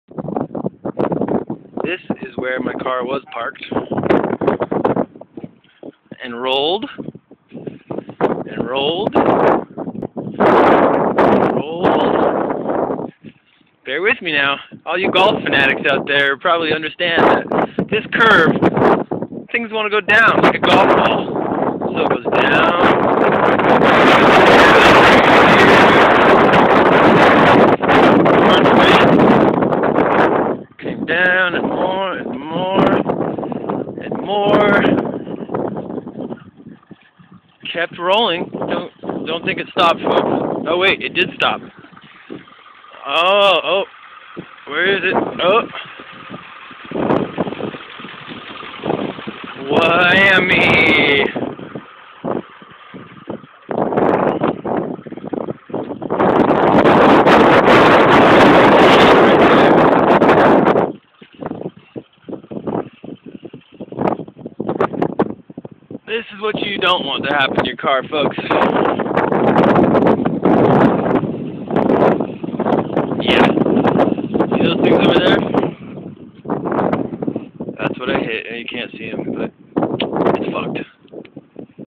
Why Cars Have Emergency Brakes
So. After a long night at work, I stopped at Dan's Grocery Store for some milk and cereal. Thats all I want. Movie and a bowl of cereal. Goodnite! But. when I get back to the parking lot, my car is GONE! I called 911, "MY CAR'S STOLEN!!". unbeknownst to me.